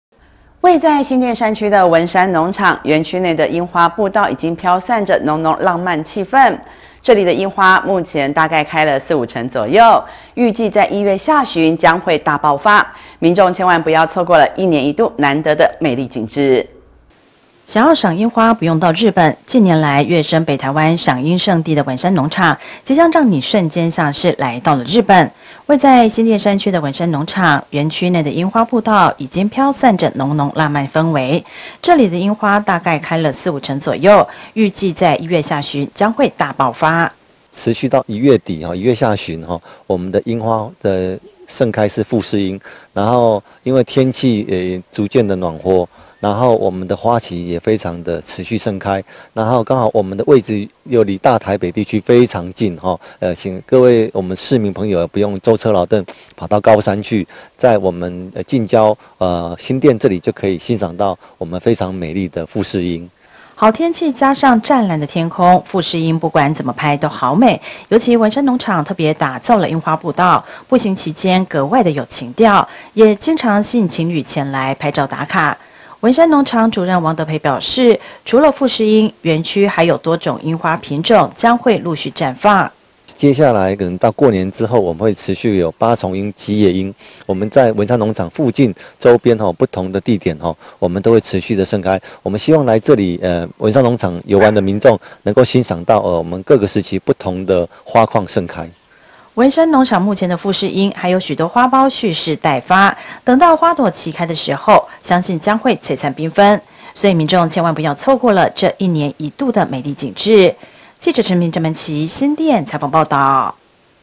文山農場櫻花報到 粉紅花朵浪漫破表
位在新店山區的文山農場，園區內的櫻花步道已經飄散著濃濃浪漫氣氛，這裡的櫻花目前大概開了四、五成左右，預計在一月下旬將會大爆發，民眾千萬步要錯過一年一度難得的美麗景致。